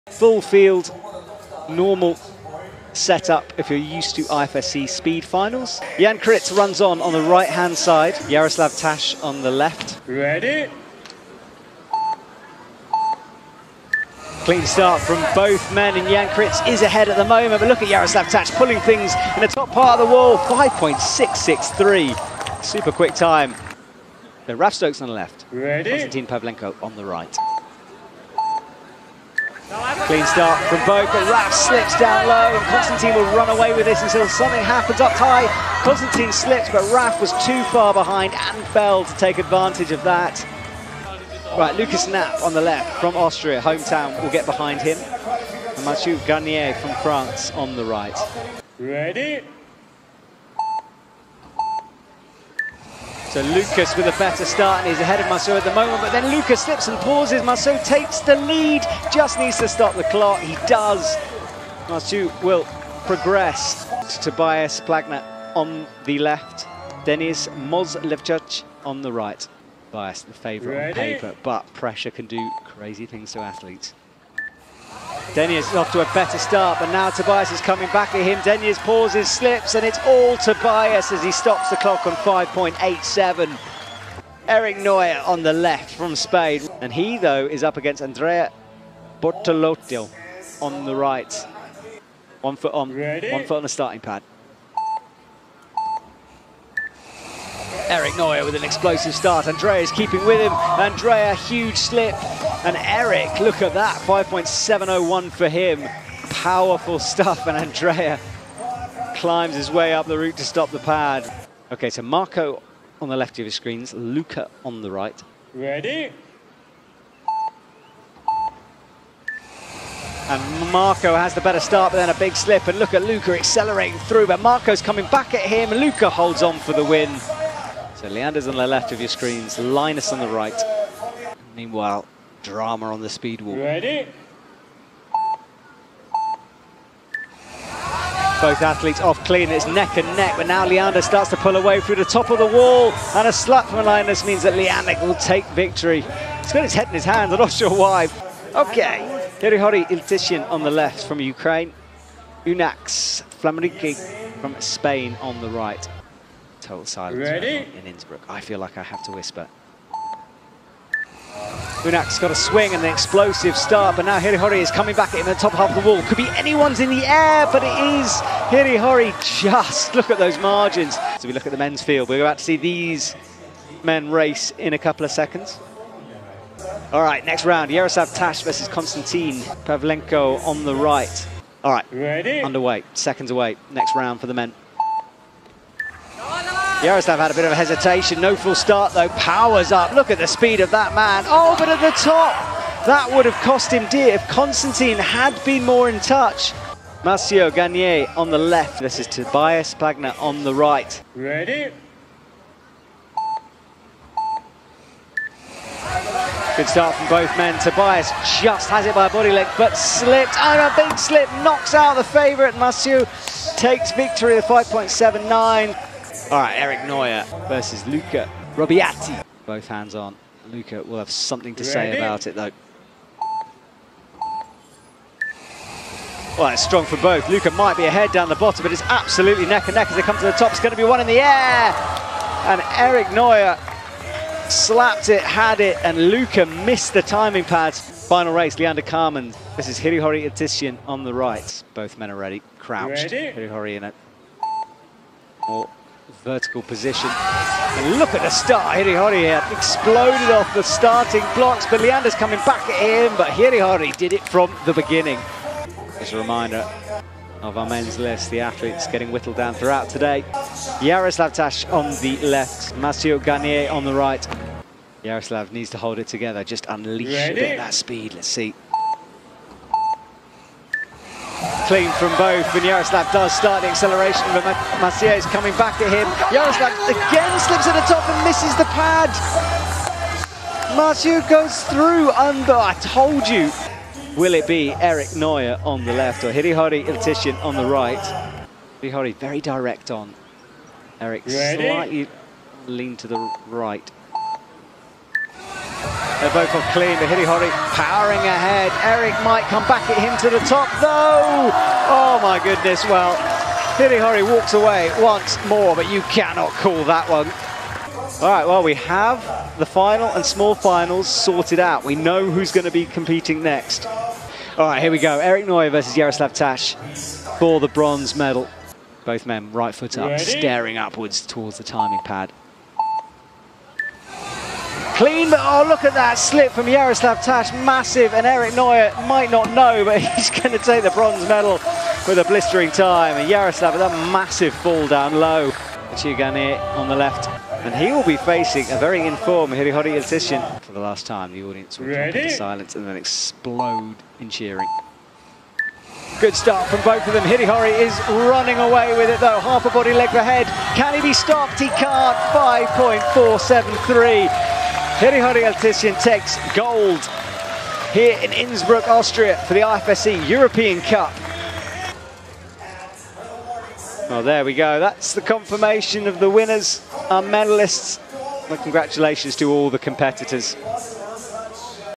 European Cup Speed Finals | Innsbruck | 2023 | Mens
IFSC Europe - Continental Cup (S) - Innsbruck (AUT) 2023
12 June - 12 June 2023

0:00 1/8
2:51 1/4
4:15 1/2
5:14 Small Final
5:35 Big Final
6:01 Outro